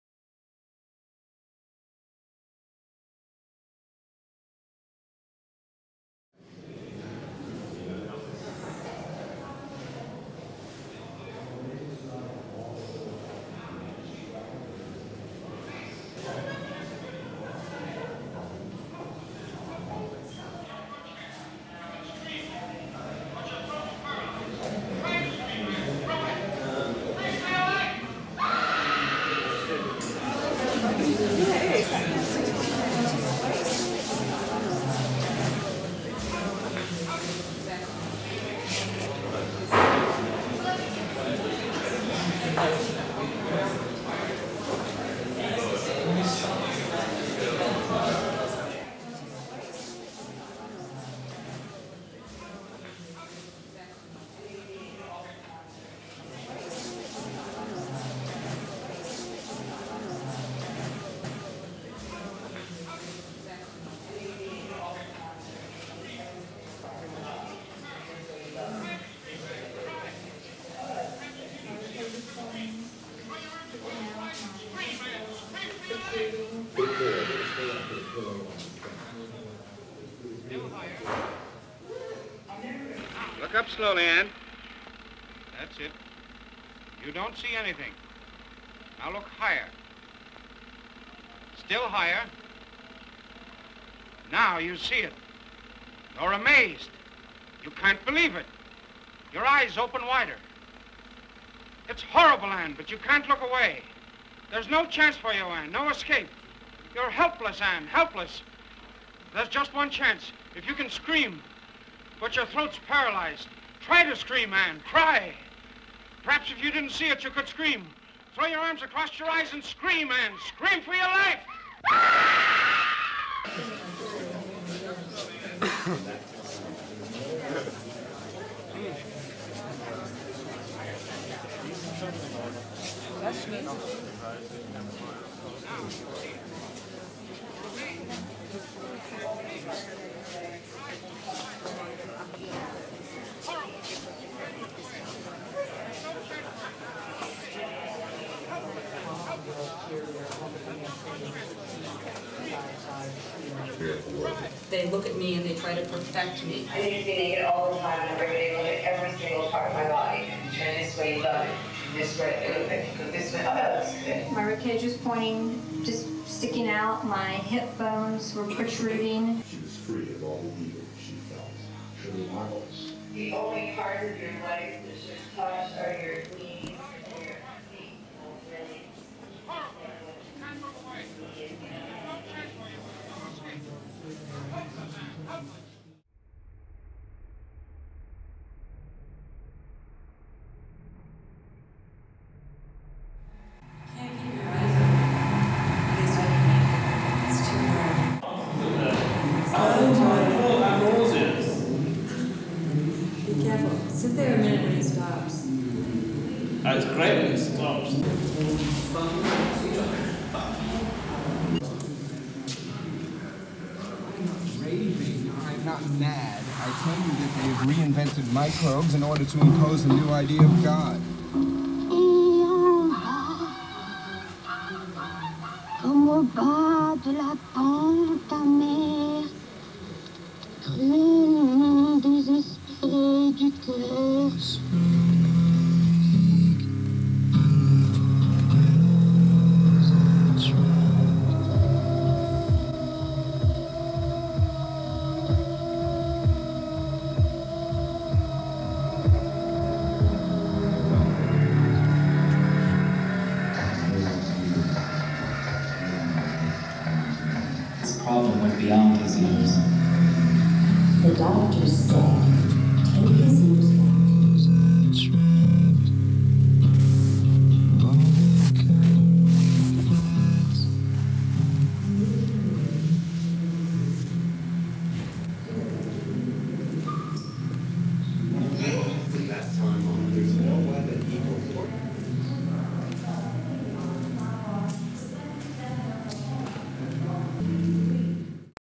CREATIVE CAP 7 3 12 22
Media installations by Beth B. Multifaceted installations include performance, digital video, sculpture, painting and audio.